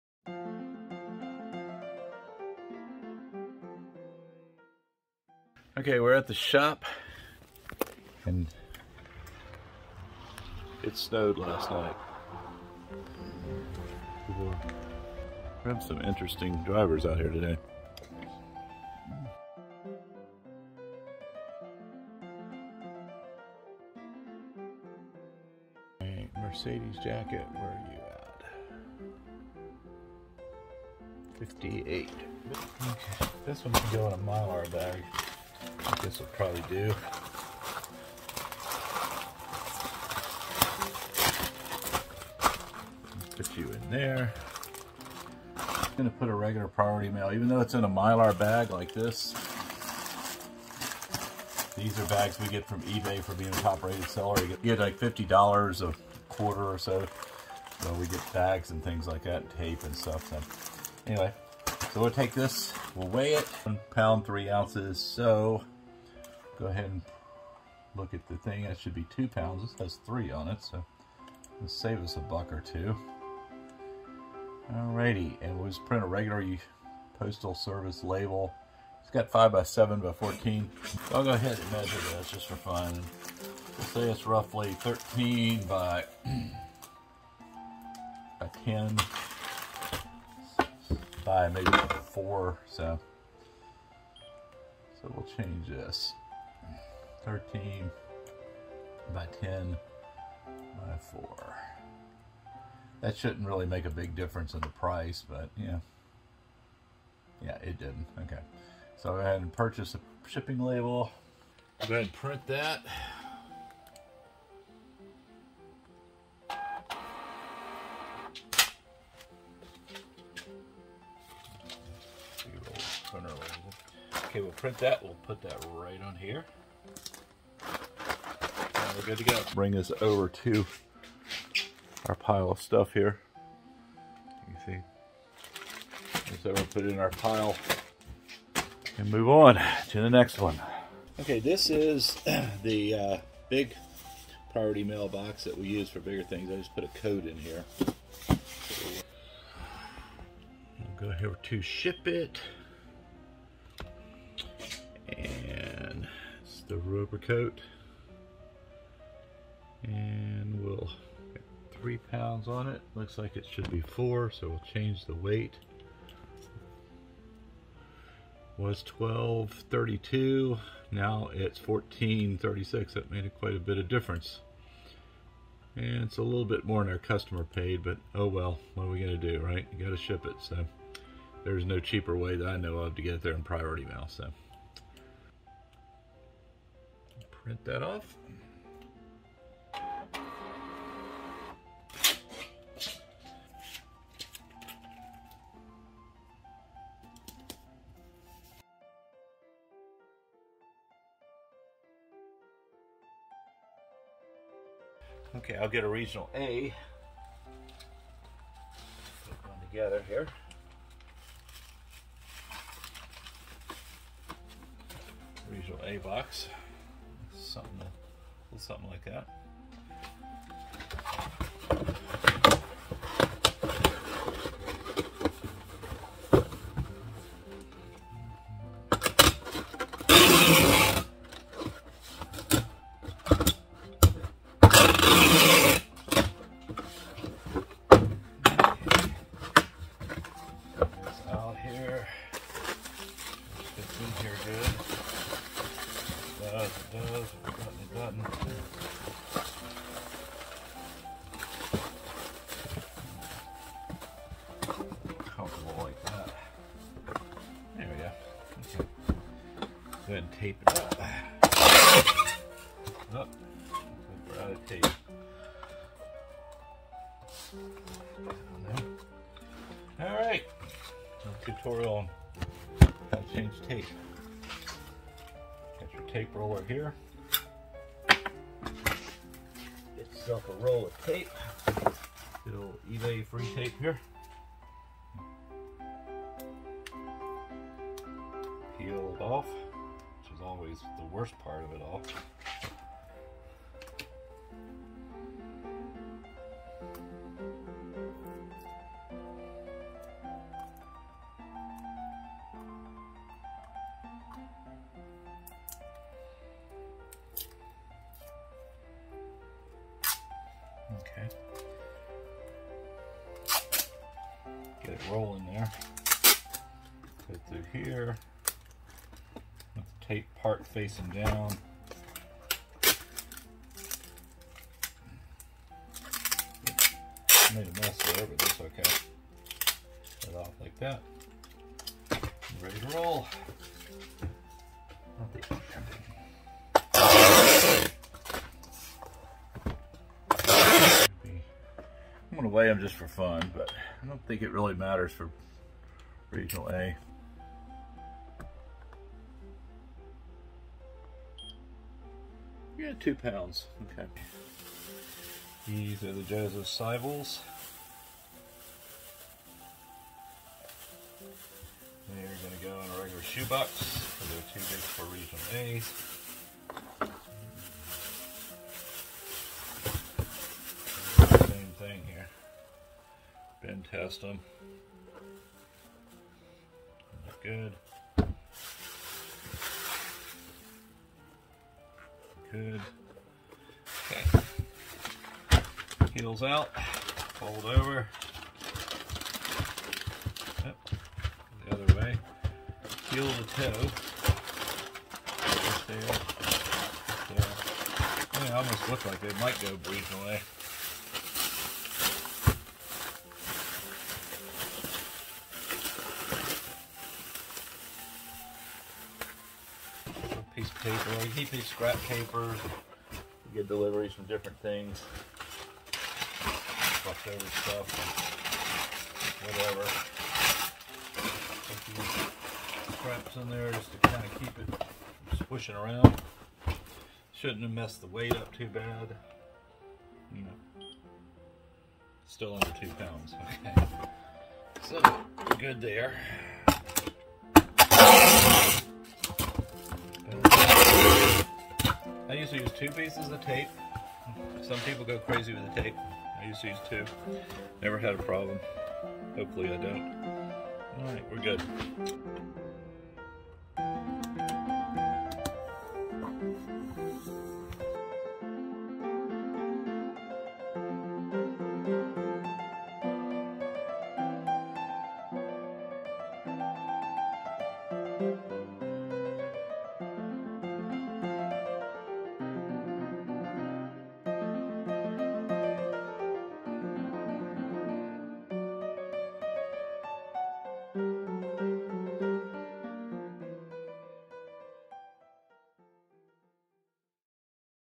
What Sold Monday: Packing and Shipping Orders for eBay
Dave goes to the shop and packs up 4 items sold on Monday.